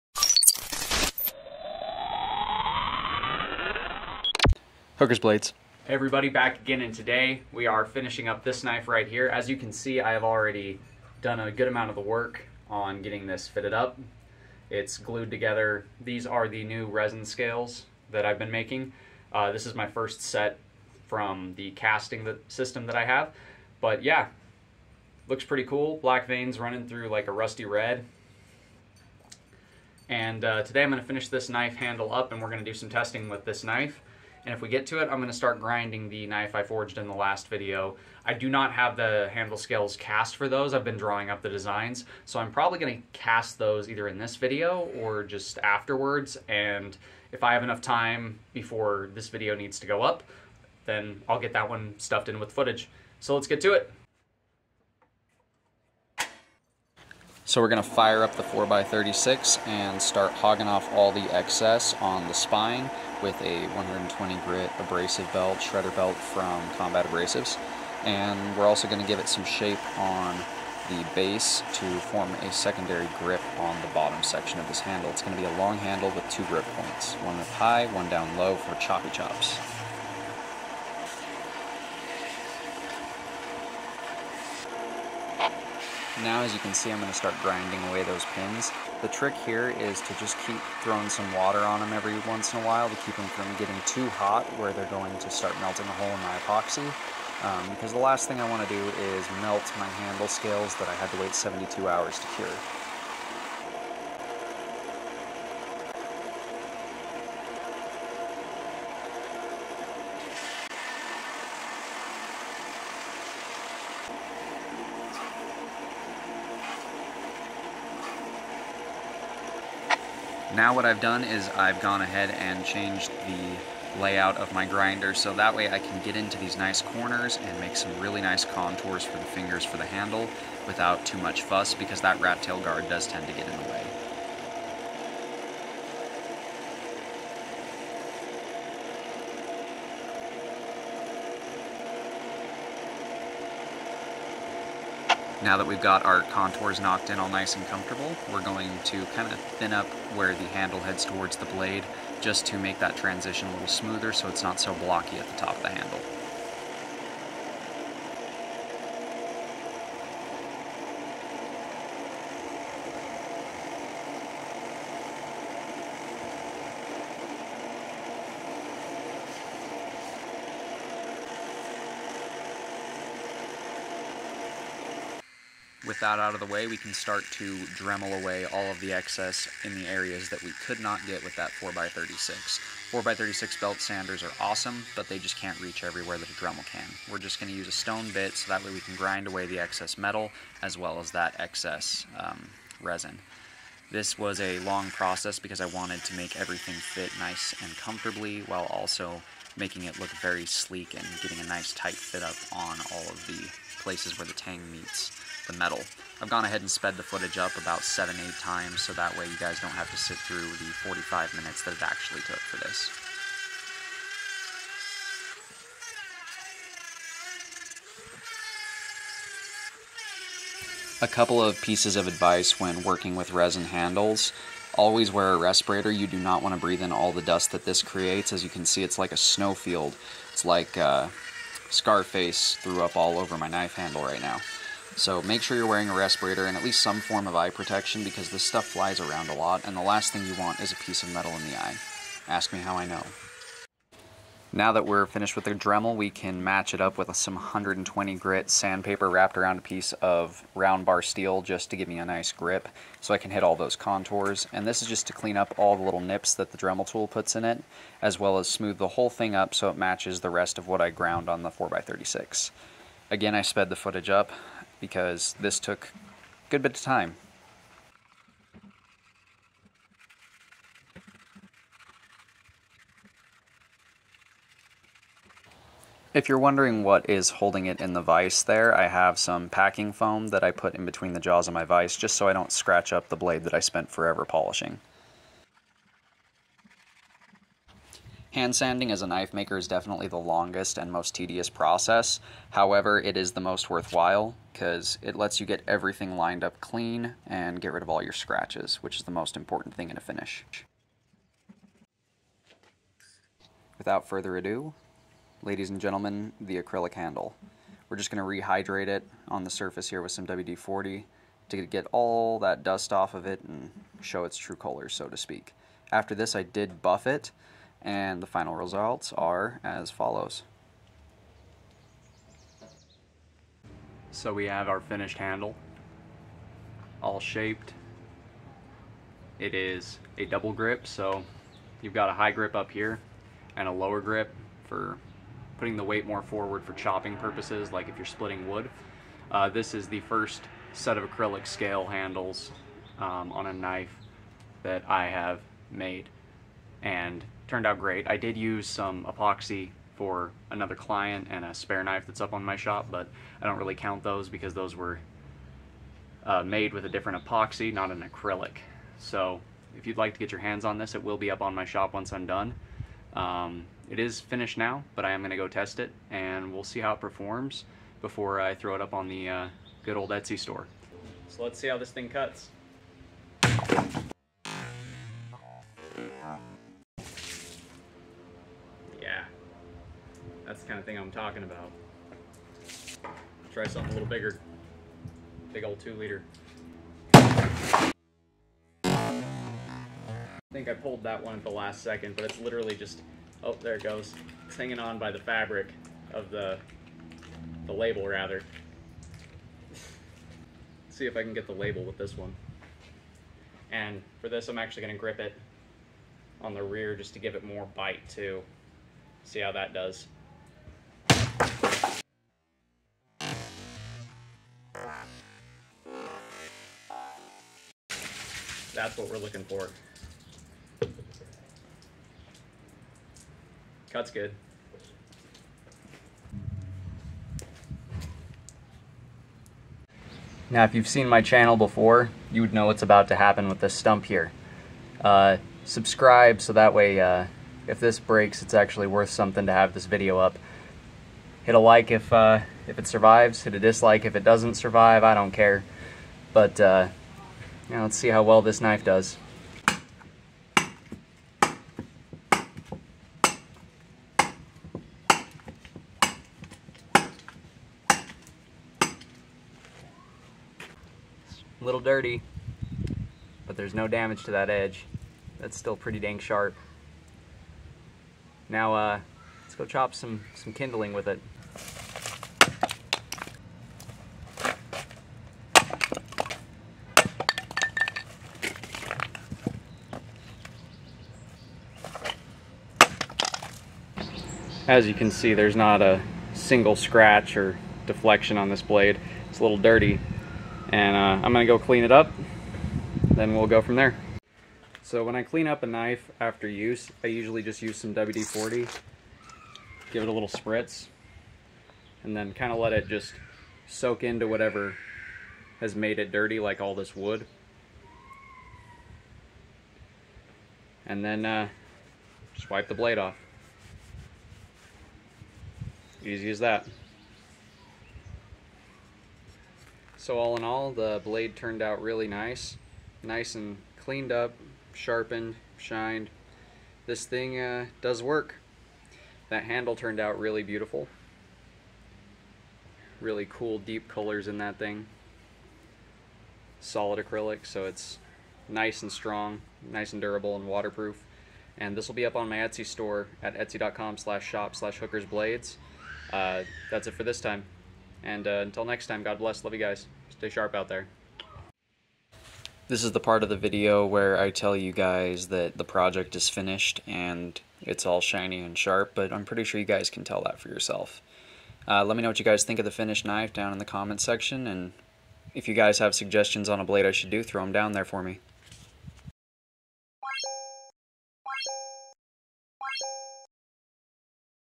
How To Finish an Epoxy Resin Handle - Finishing and Testing the WW3 Kukri Knife
in this video I show the process from sanding to grinding and everything in between a finishing an epoxy resin handle on a knife.
check out what I sell at
https//Etsy.com/shop/hookersblades